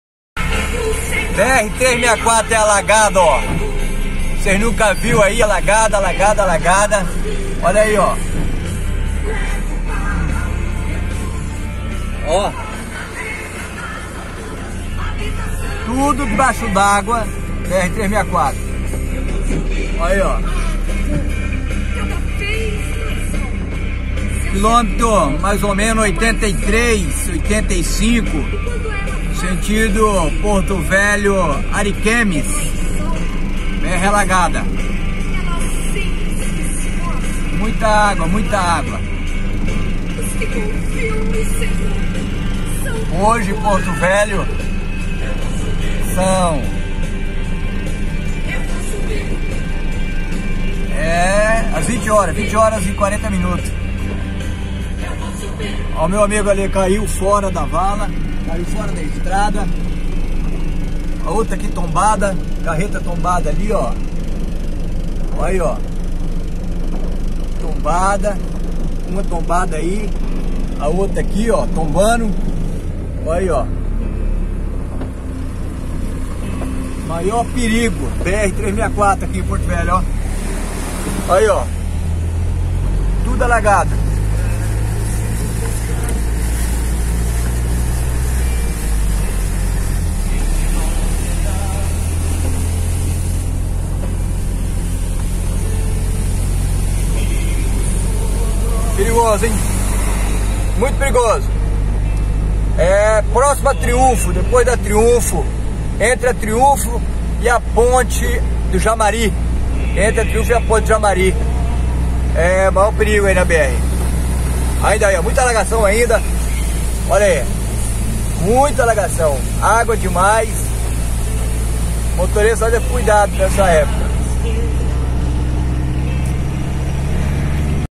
Chuva deixa Br 364 alagada
Chuva alaga Br e vários caminhões tombado